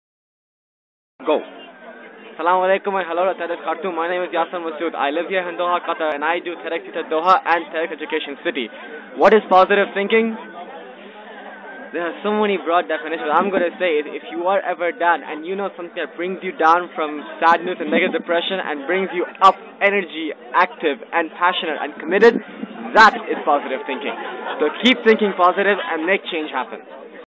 Yasser Masood Khan ‏ of TEDxEducationCity to TEDxKhartoum: What's Positive Thinking?
During TEDxSummit, Yasser Masood Khan ‏ of TEDxEducationCity in Doha shares his thoughts about TEDxKhartoum 2012 theme: Positive Thinking?